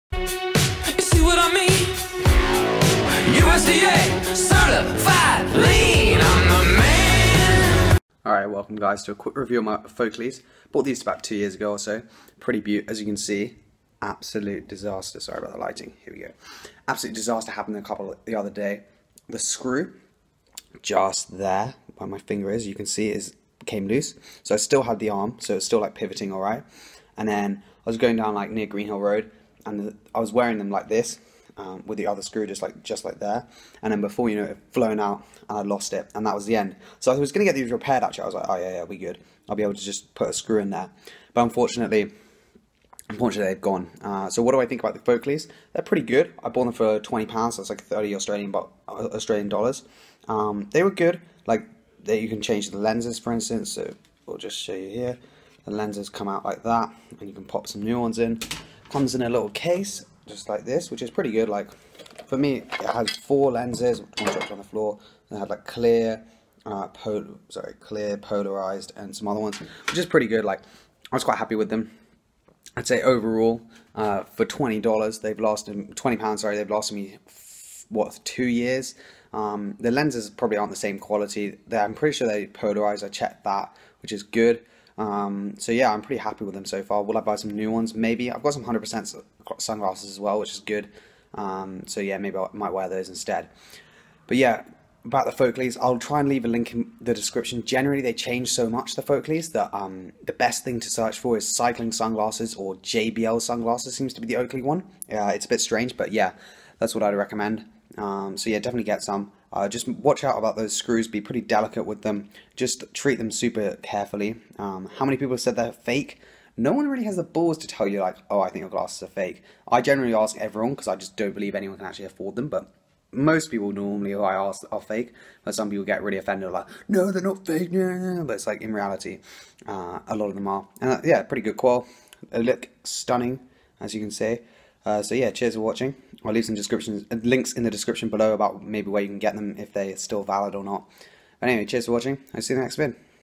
Foakleys 2 Years On - Review
How I got on with my Fake Oakleys after two years with them. Strava